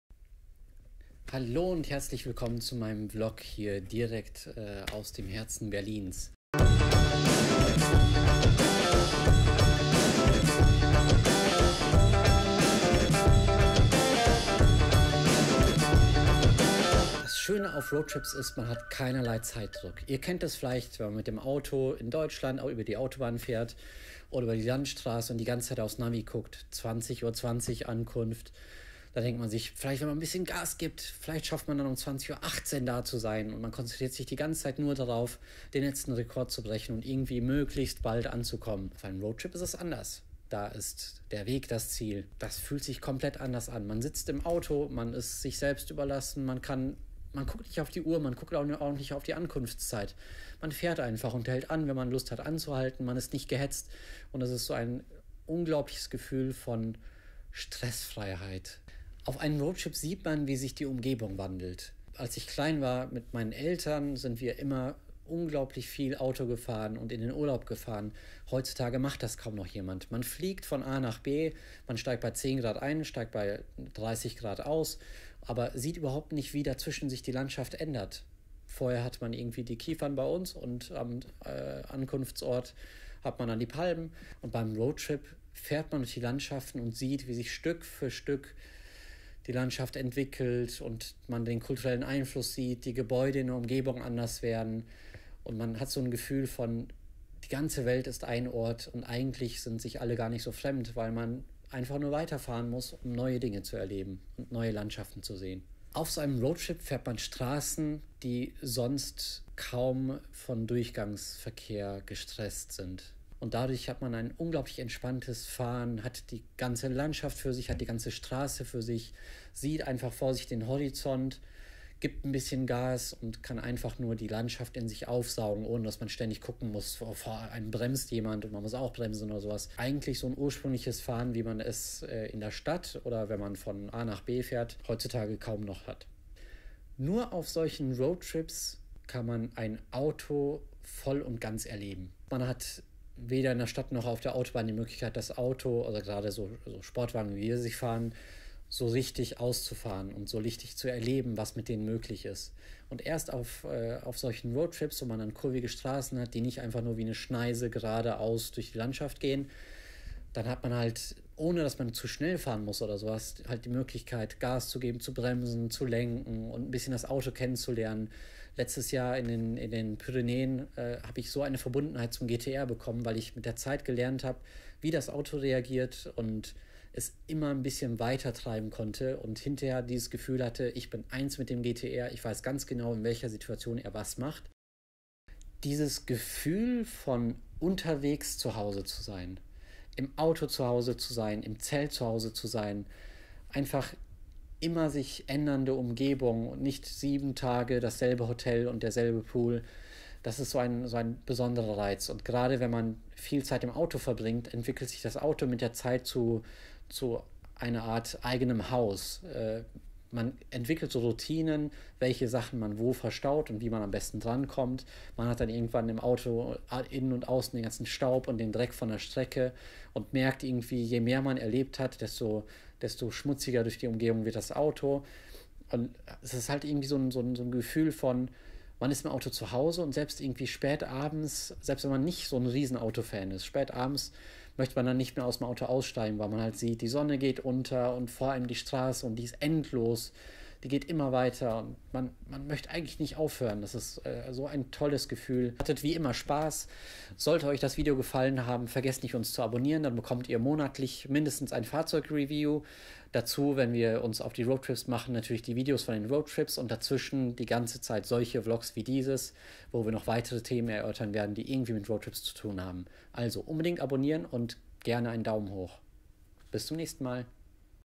Die Top 5 Gründe einen ROADTRIP zu machen!
Nachdem ich zuletzt erzählt habe, was die 5 Dinge sind, die bei jedem Roadtrip passieren, möchte ich euch diesmal erzählen, was die Top 5 besten Dinge sind, die jeden Roadtrip aus machen.
Denn ein Urlaub in den Bergen oder in schönen Landschaften, den man größtenteils auf der Straße verbringt, hat durchaus anderes zu bieten als ein Pauschalurlaub. Statt mit dem Flieger zwischen zwei sehr unterschiedlichen Welten zu wechseln, erlebt man bei einem Roadtrip den langsamen kulturellen und landschaftlichen Wandel der Umgebung.